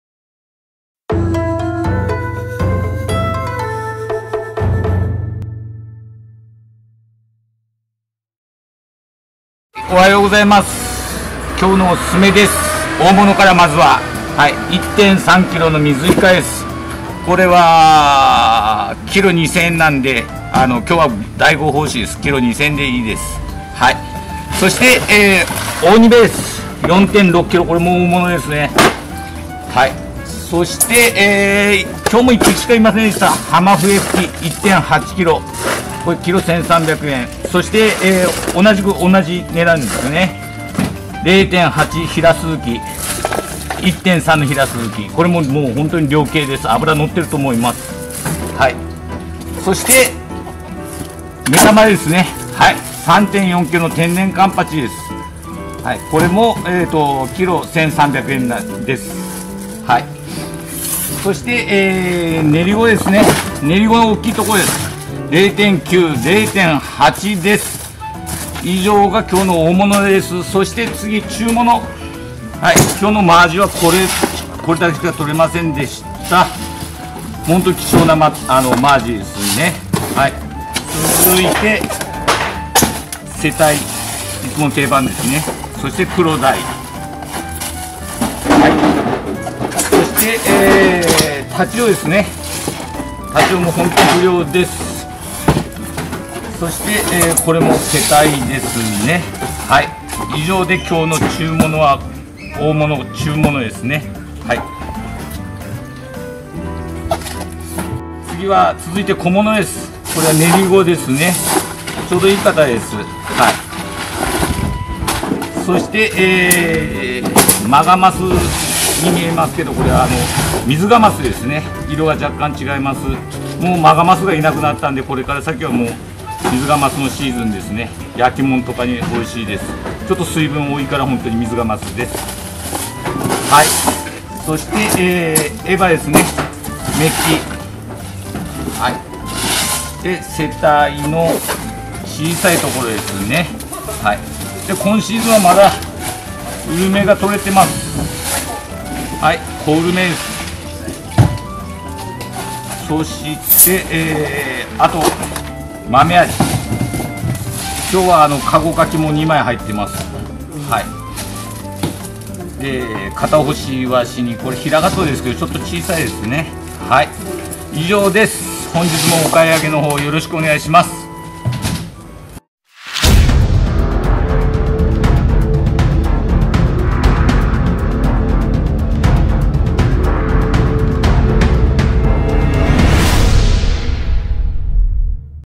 大物！天然カンパチ3.4キロ！ ～延岡の海で獲れた魚たち～
■凄まじい大漁！！！合計2トン！獲れすぎてやばい！（太刀魚500キロ＋カマス1.5トン）

魚の鮮度や美味しさには絶対の自信があります！